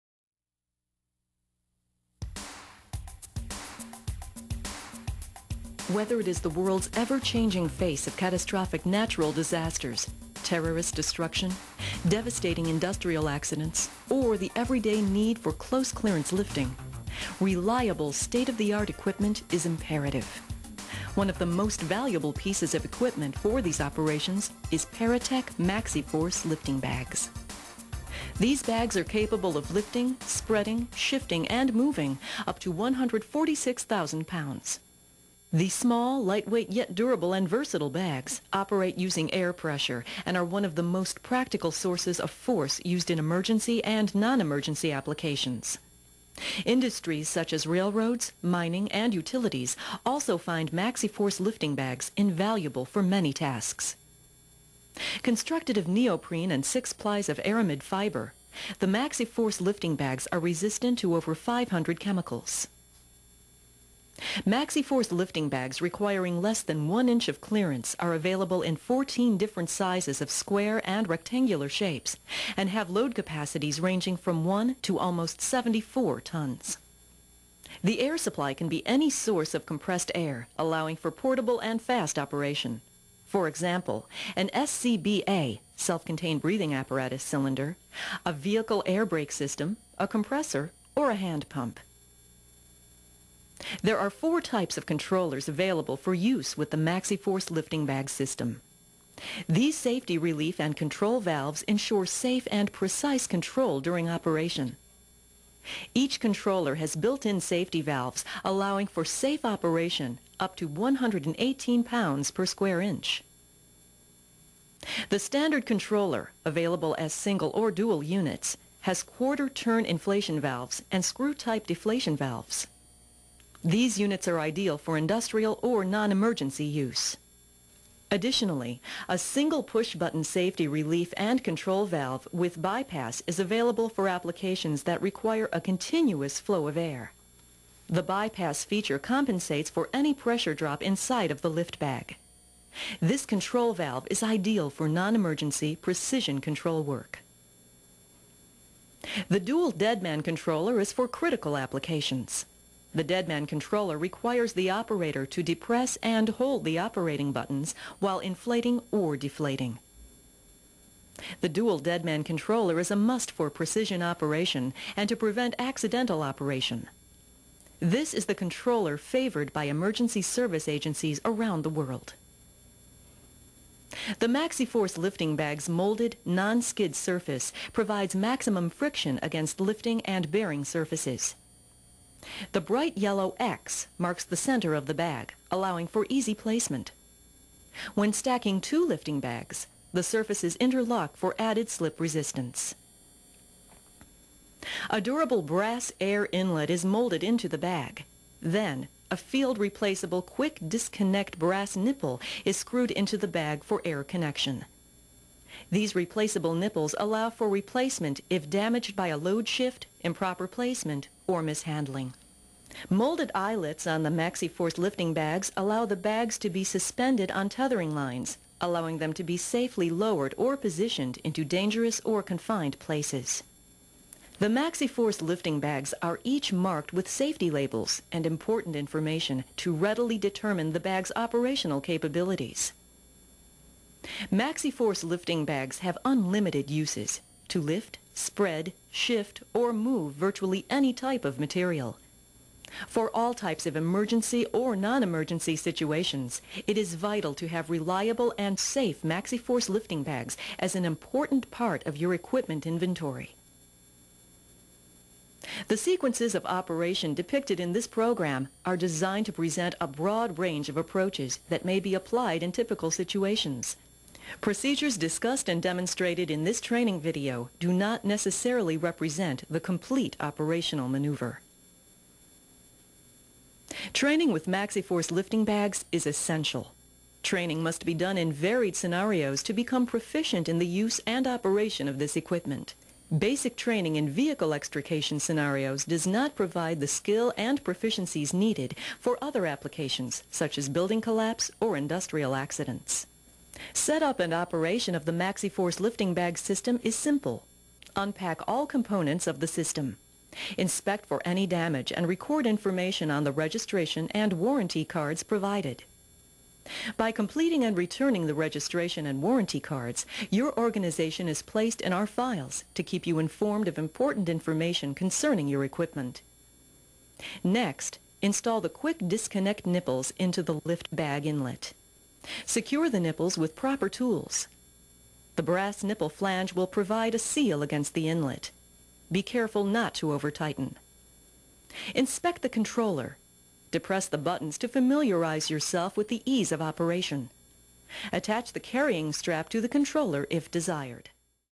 PARATECH MAXIFORCE INTRO.flv
Introdcution to use of our Maxiforce lifting bags carried on our apparatus